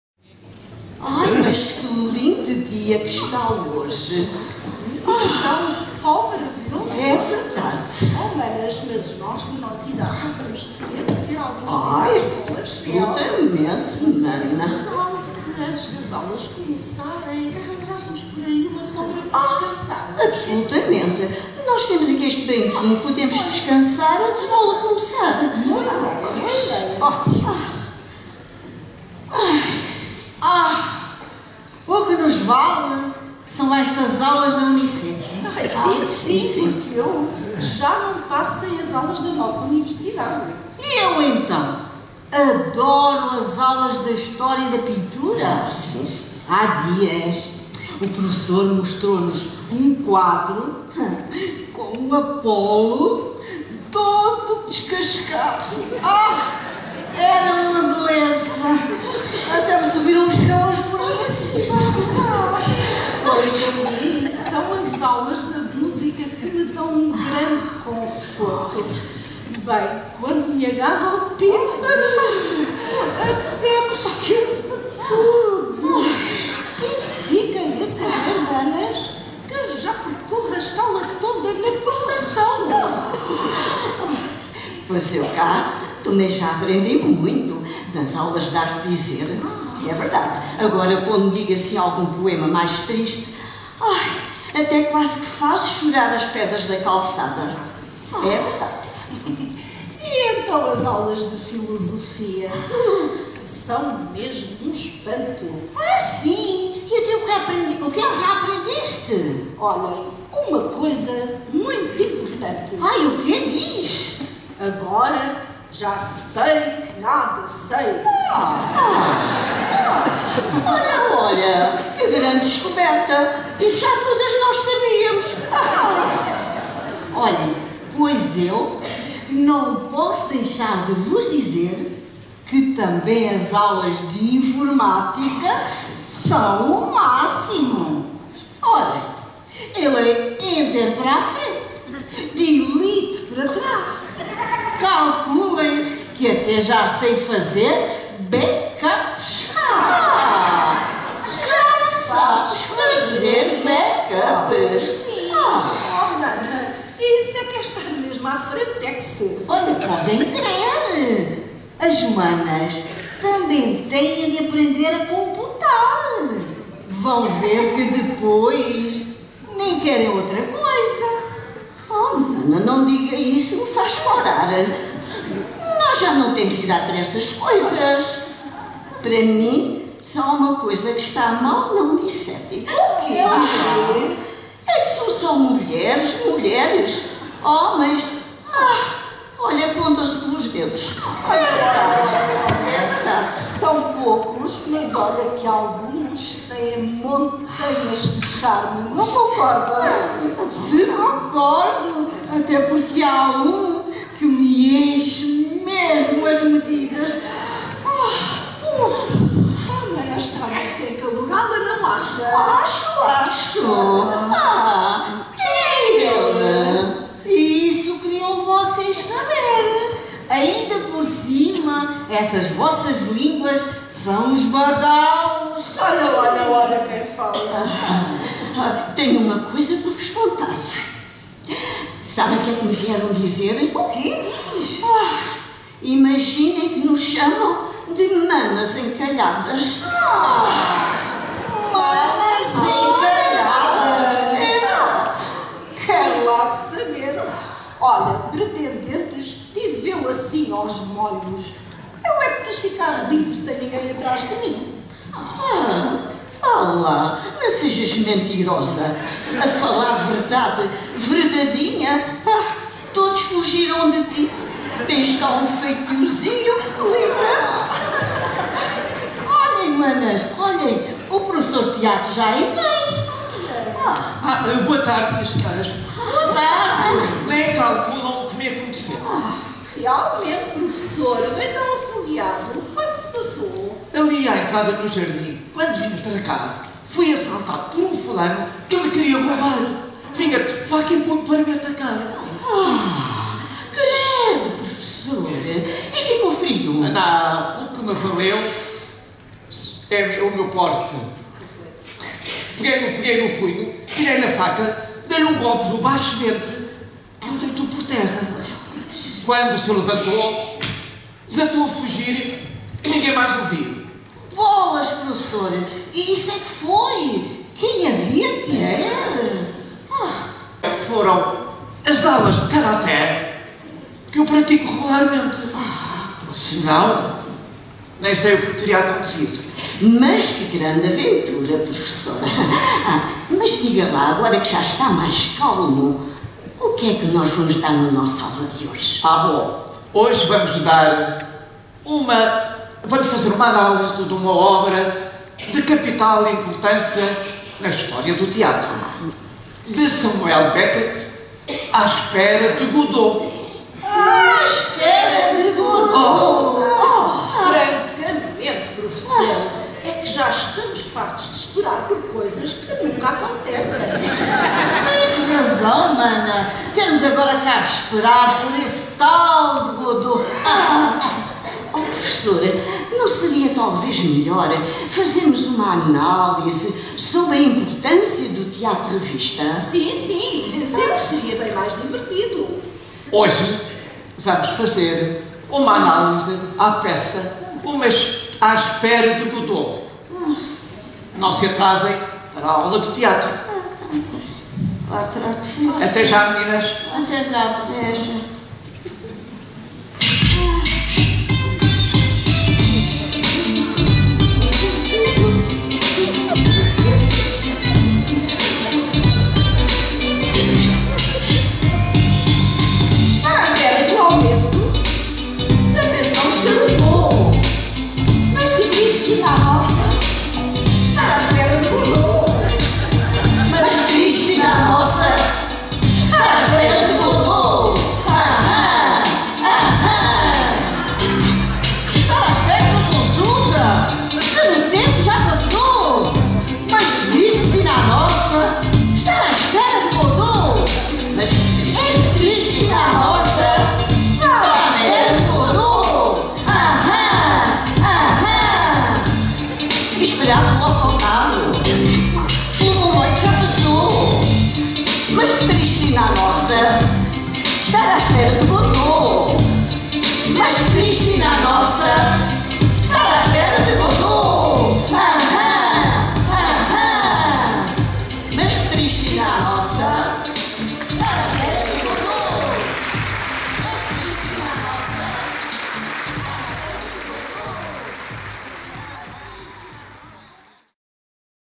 UNISETI - AS MANAS ENCALHADAS
SETÚBAL TEM DESTAS COISAS. “simoesdasilva2” - - A UNISETI – UNIVERSIDADE SÉNIOR DE SETÚBAL - APRESENTA: “VAMOS AO TEATRO! - MÚSIC - TEATRO - POESIA”.   - FÓRUM LUISA TODI, 14 DE MAIO DE 2019